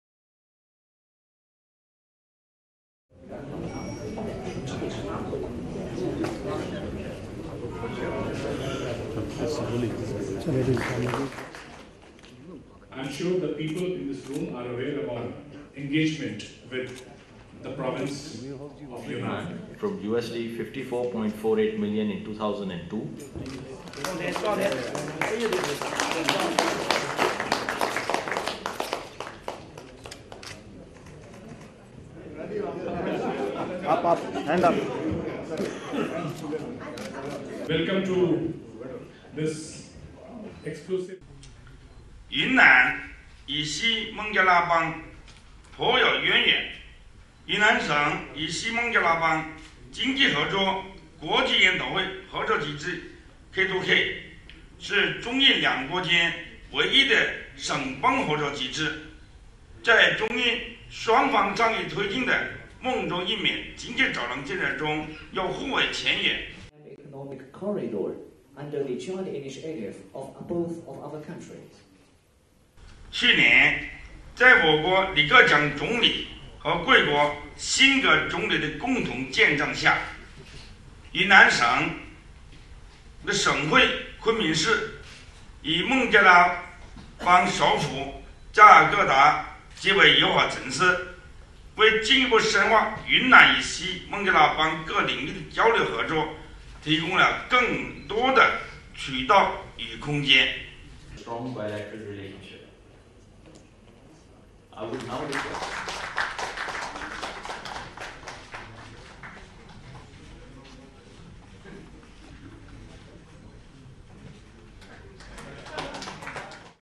Governor of China's Yunnan Province Hails Economic Ties With India's Kolkata
Governor of China's Yunnan province, Li Jiheng during an exclusive business meet in India's eastern province of West Bengal hails the economic engagement between Kolkata and Yunnan.